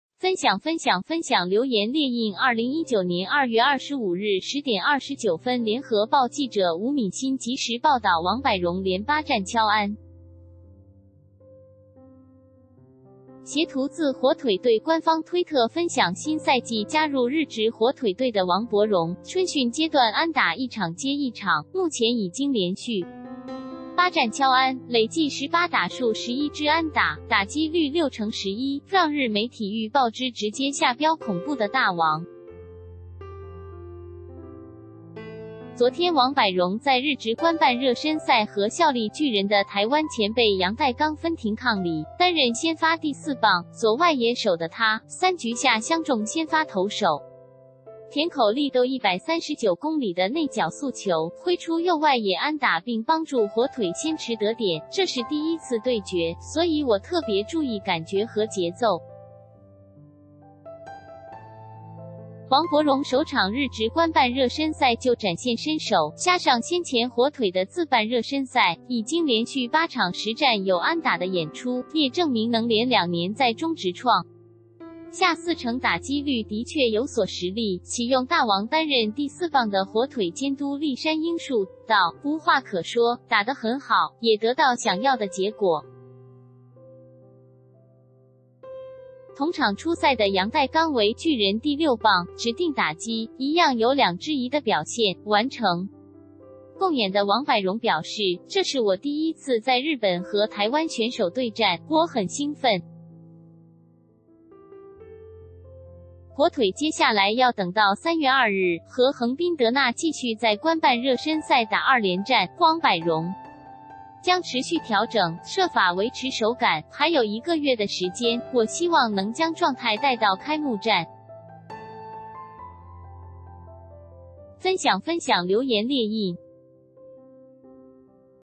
日職／王柏融8戰打擊率破6成 日媒：恐怖的大王 | 聯合新聞網
+分享分享分享留言列印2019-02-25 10:29聯合報 記者吳敏欣╱即時報導 王柏融連8戰敲安。 擷圖自火腿隊官方推特 分享 新賽季加入日職火腿隊的王柏融，春訓階段安打一場接一場，目前已經連續8戰敲安，累計18打數11支安打、打擊率6成11，讓日媒「體育報知」直接下標「恐怖的大王」。昨天王柏融在日職官辦熱身賽和效力巨人的台灣前輩陽岱鋼分庭抗禮，擔任先發第4棒、左外野手的他，3局下相中先發投手田口麗斗139公里的內角速球，揮出右外野安打並幫助火腿先馳得點，「這是第一次對決，所以我特別注意感覺和節奏。」王柏融首場日職官辦熱身賽就展現身手，加上先前火腿的自辦熱身賽，已經連續8場實戰有安打的演出...